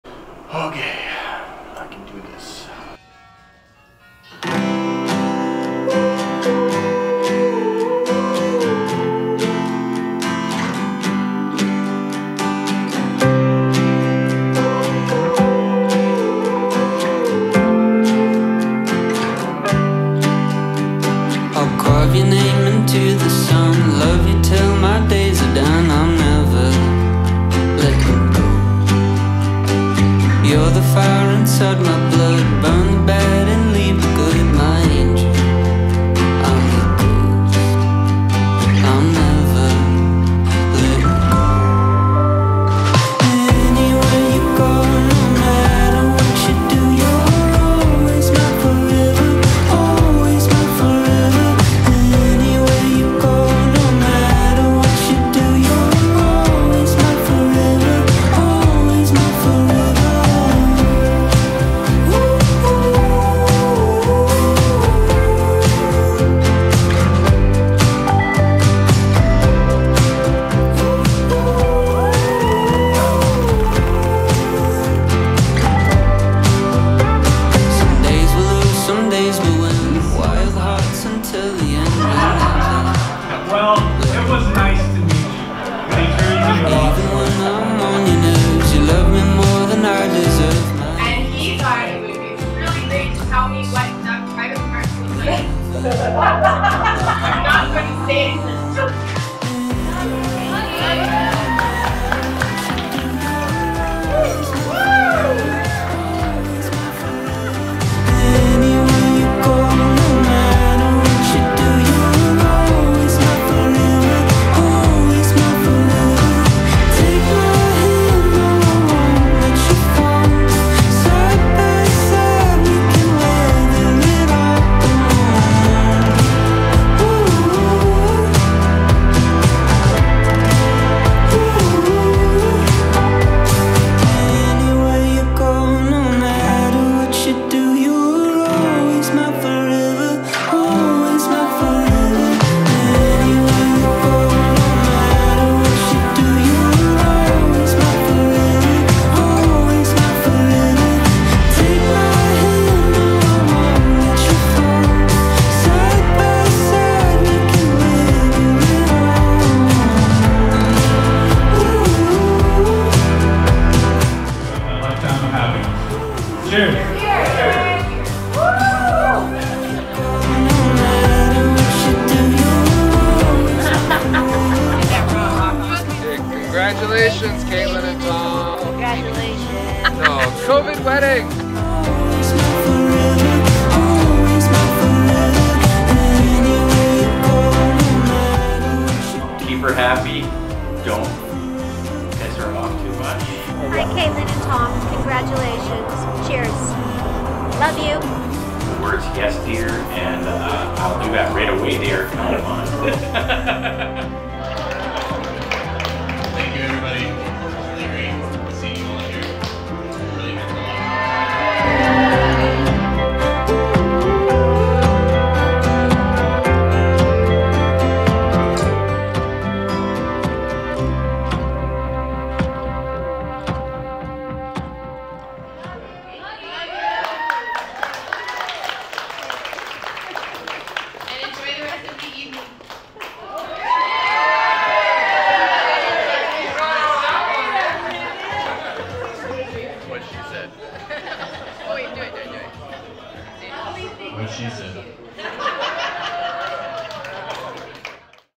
Hycroft wedding highlights I Intimate wedding I Caitlyn & Tom I Vancouver, BC 2020
Wedding highlights  video of an intimate wedding of Caitlyn & Tom from stunning heritage looking venue which Hycroft Mannor for sure is. Unique looking venue in the middle of Vancouver hosted romantic and intimate wedding of a couple during Corona virus pandemic in 2020. Even under those consequences, their COVID ceremony was beautiful.